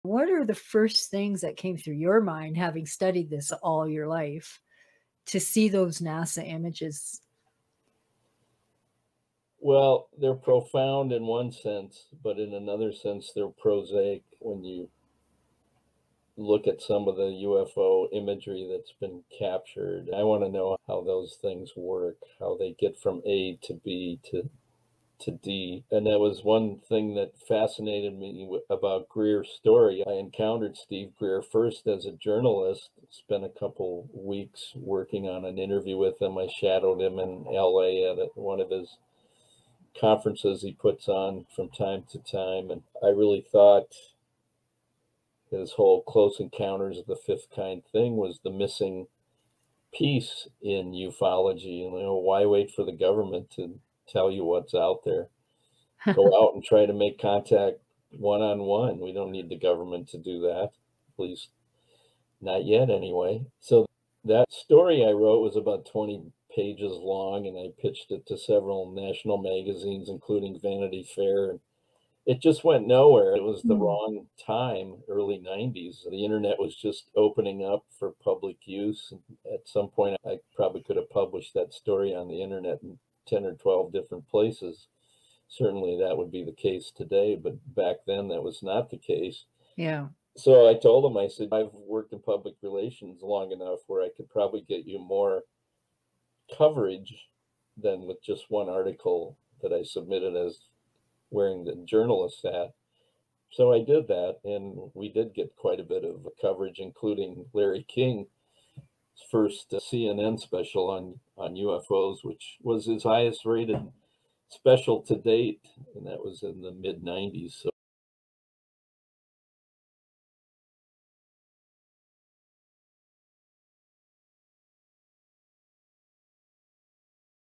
James Webb Space Telescope and Steven M Greer
Martin Keller describes the extraordinary work of Steven Greer and his thoughts about NASA's James Webb Space Telescope.